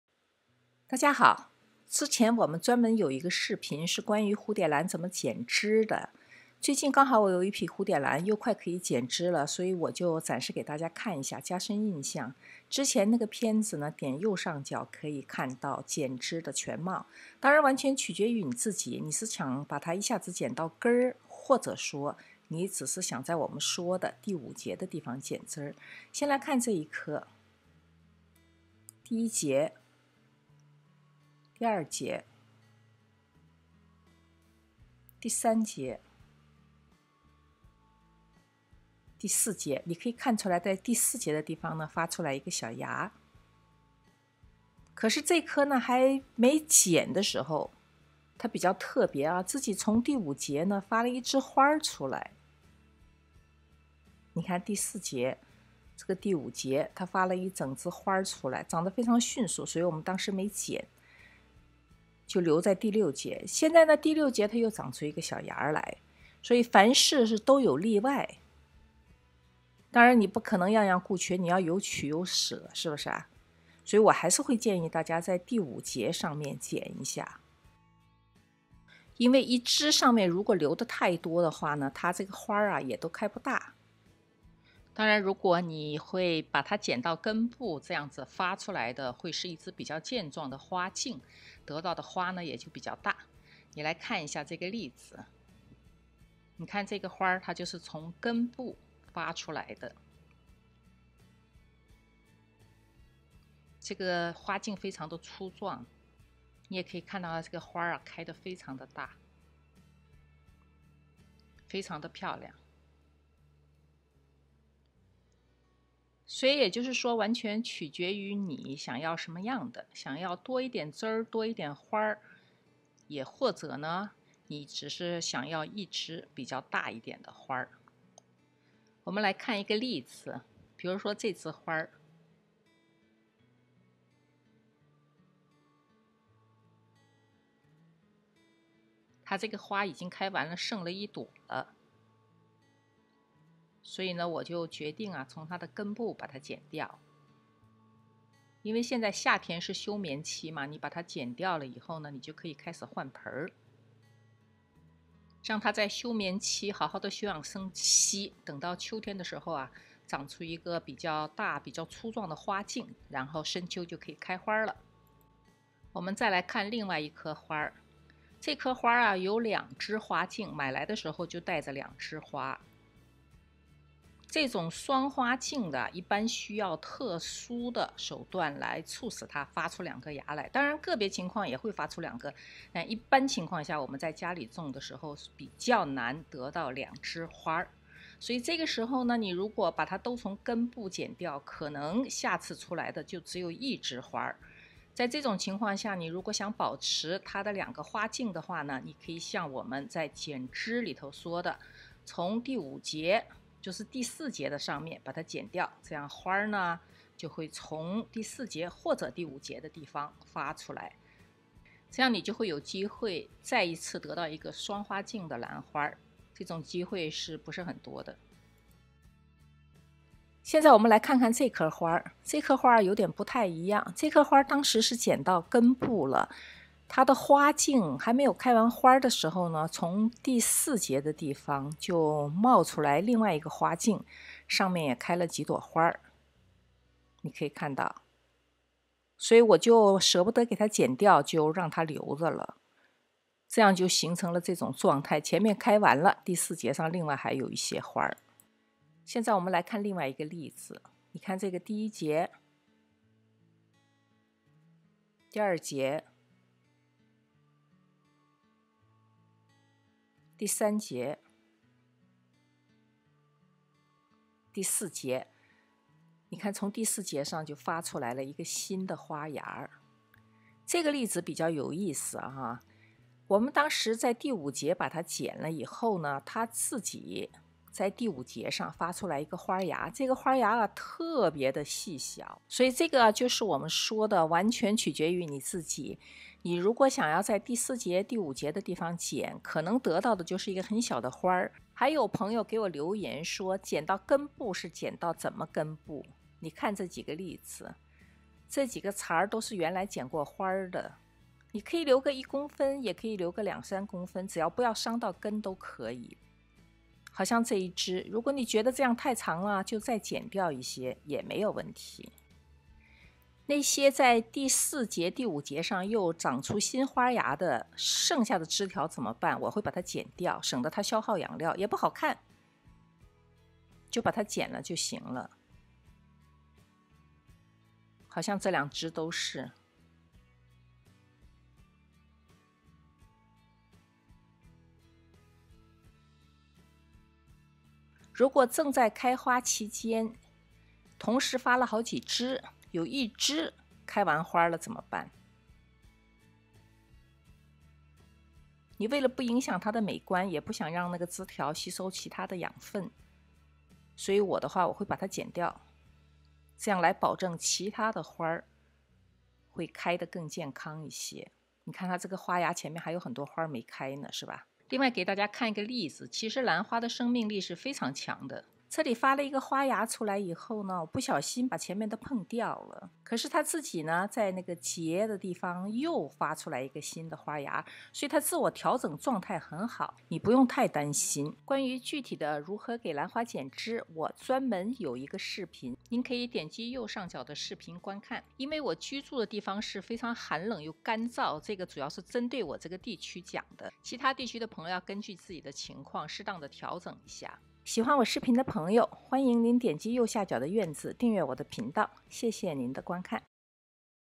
【彬彬有院】花 •新手养蝴蝶兰第三步，你必须知道的蝴蝶兰剪枝那些事
我之前有一个专门的关于兰花剪枝的视频，这里补充了几个例子给新手看看，让大家能更好的理解。
我还要再说一遍，我的这些视频是我的经验分享，而我所在地区是寒冷干燥的地方。如果你所在地区有不同的气候，要根据情况适当调整。
爱好兰花使得我们聚在一起，我会继续在这里分享我的经验。